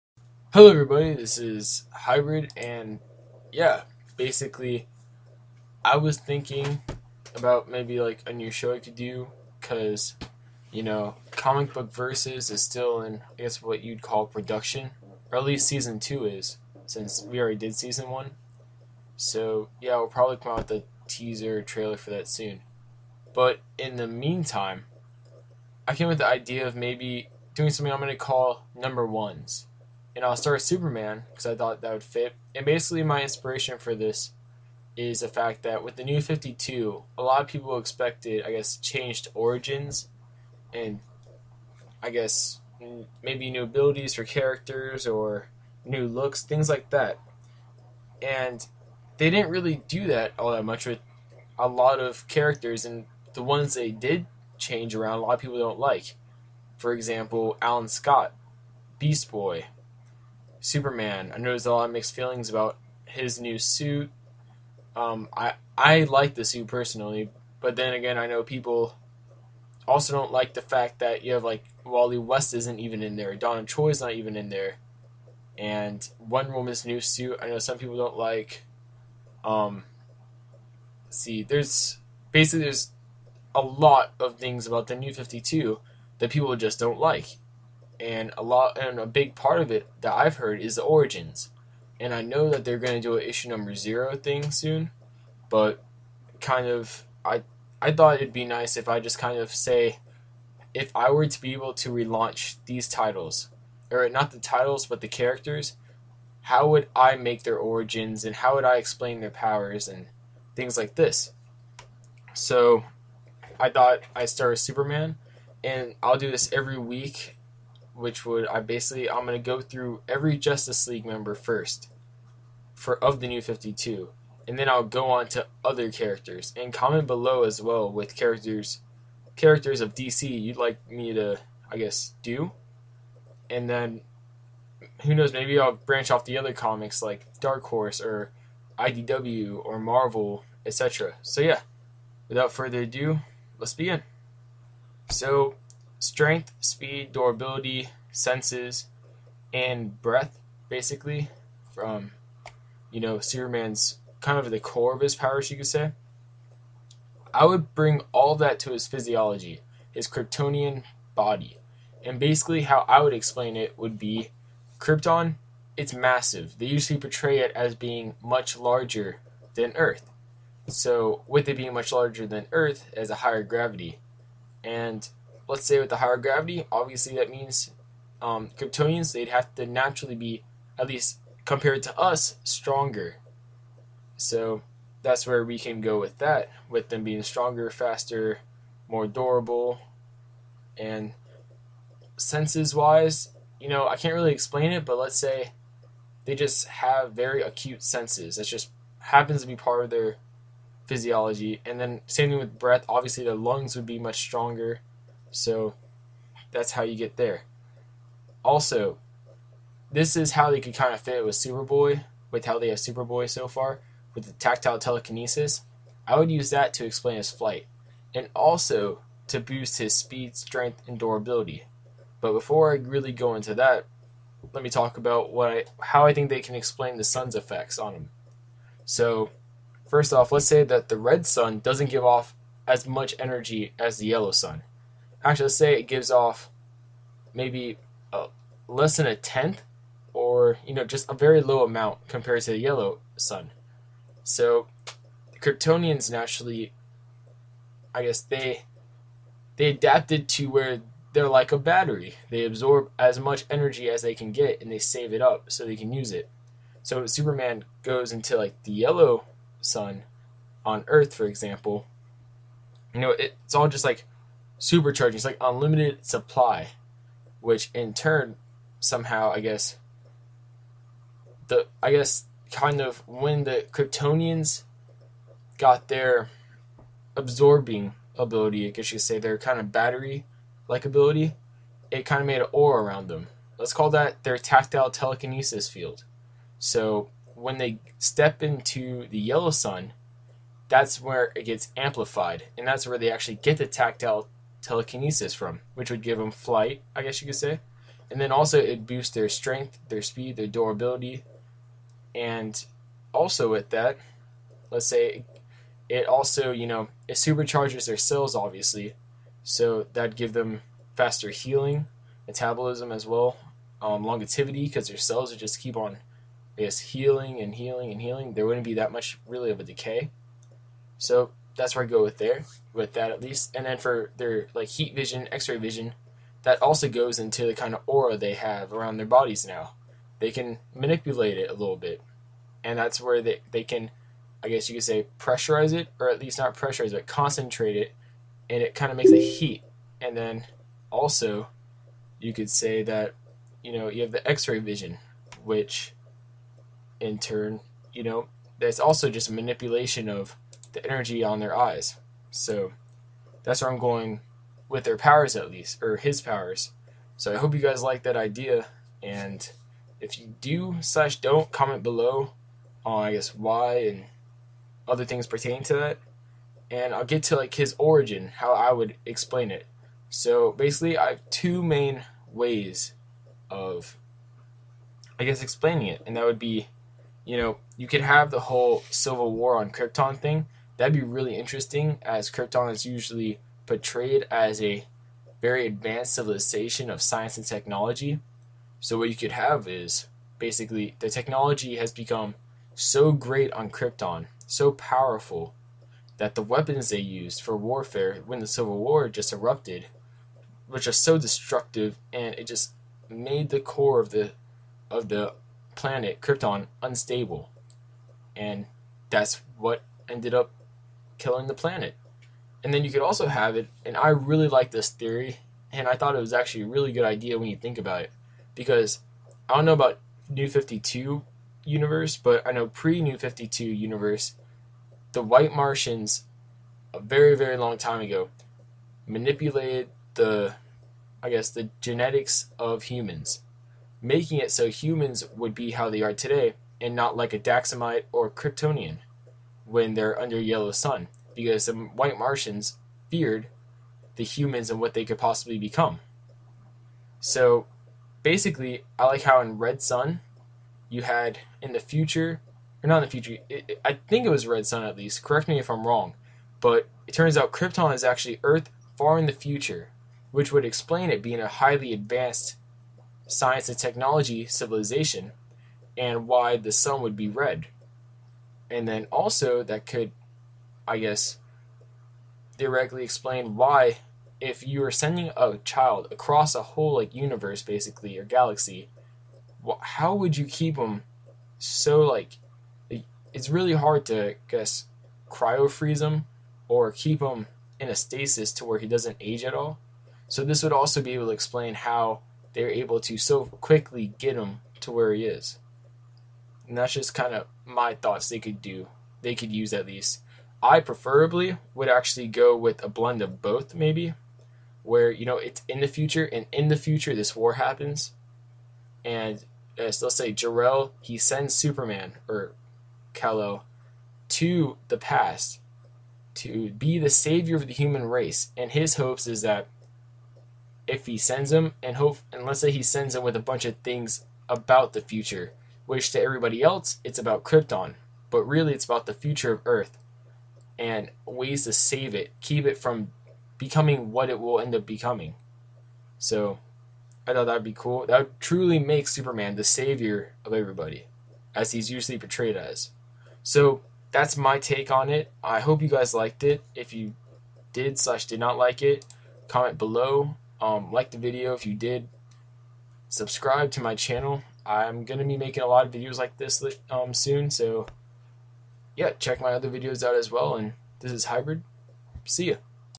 #1: Superman
My new weekly series where I say how I would explain Superman's powers, abilities, origins, etc. if I were writing a reboot to the Superman comics.